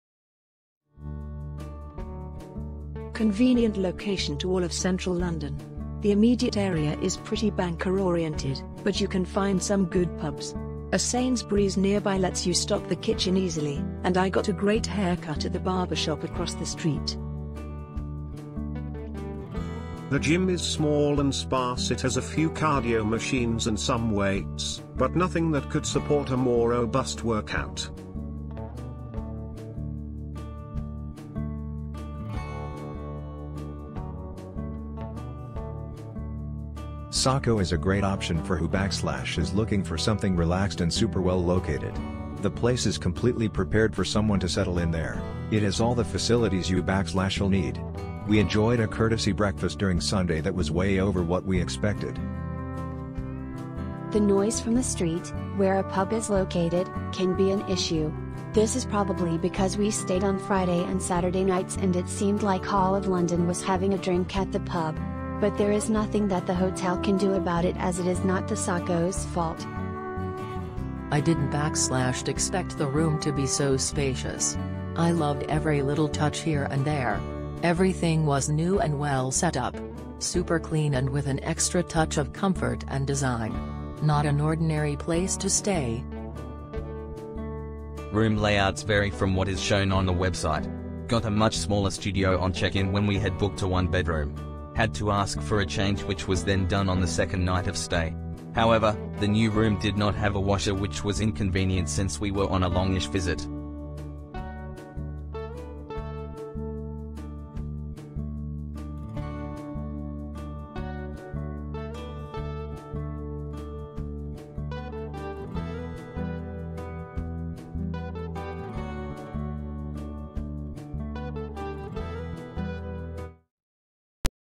SACO - The Cannon 4 ⭐⭐⭐⭐ | Reviews real guests Hotels in London, Great Britain
SACO - The Cannon ⭐⭐⭐⭐ | Reviews real guests Hotels in London, Great Britain

SACO - The Cannon is set in London, 0.7 mi from Tower of London. Tower Bridge is 0.9 mi from the property. All units include a dining area and a flat-screen TV. There is also a kitchen, fitted with an oven. A microwave and kettle are also offered. Every unit features a private bathroom with free toiletries and a hair dryer. Towels are provided. Free breakfast is served every Sunday morning in the lounge area between 10:00 and 10:30. This is available to all guests. There is a weekly cleaning service provided. Somerset House is 1.1 mi from SACO - The Cannon, while Lyceum Theater is 1.1 mi away. The nearest airport is London City Airport, 6.2 mi from SACO - The Cannon. City of London is a great choice for travelers interested in history, city trips and city walks.